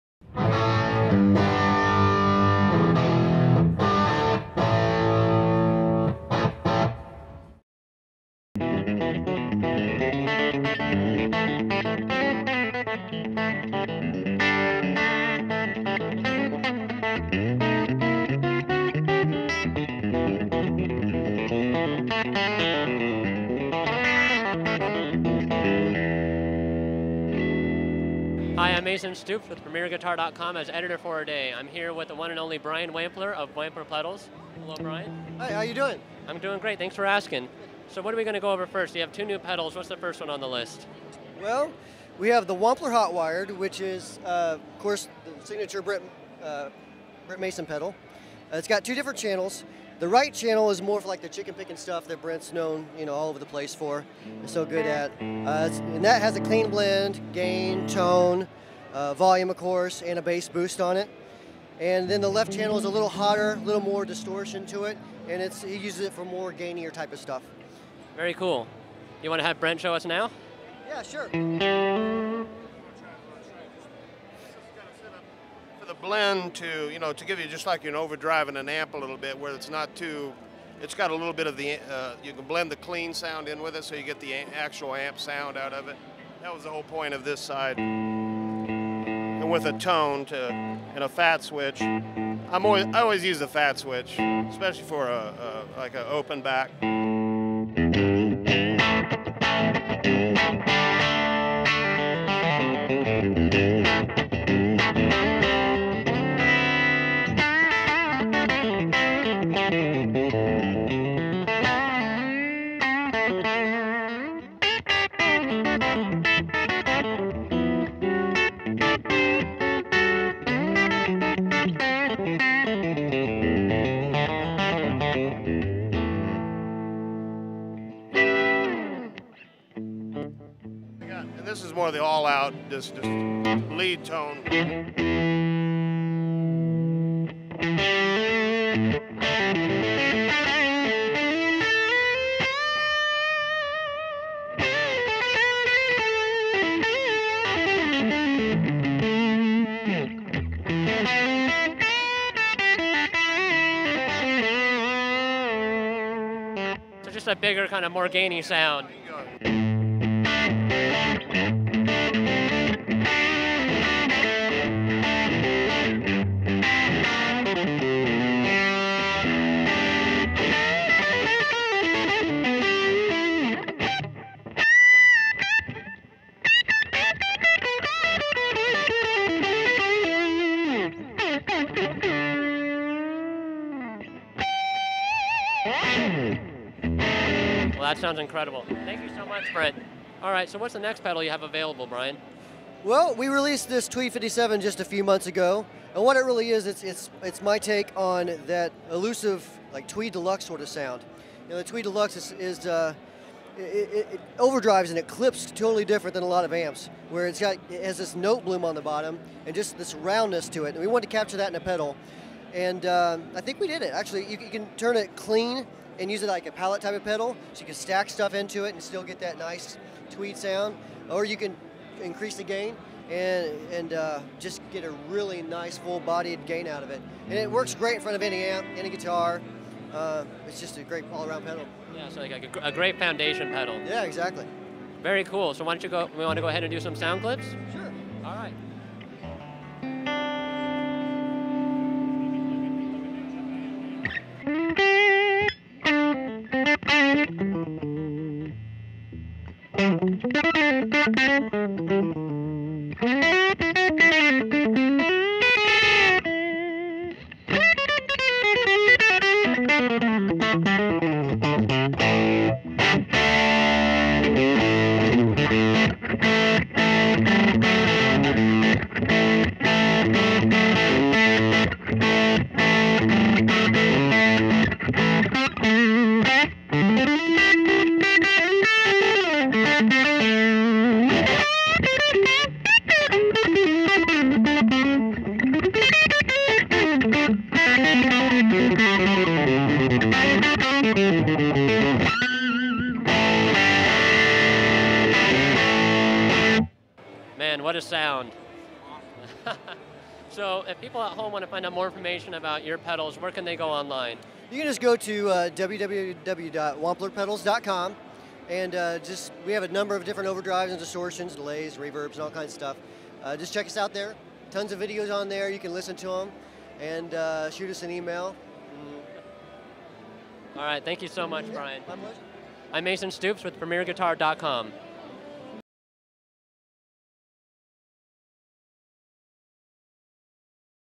Summer NAMM '12 - Wampler Pedals Brent Mason Signature Hot Wired and Tweed '57
PG's Editor for a Day winner Mason Stoops is On Location in Nashville, TN, for the 2012 Summer NAMM Show where he visits the Wampler Pedals booth. In this segment, we get to see and hear a demos of Brent Mason's Signature Hot Wired pedal and the Tweed '57 pedal.

For more Summer NAMM 2012 video demos or to watch one of Premier Guitar's 1700 other clips online, be sure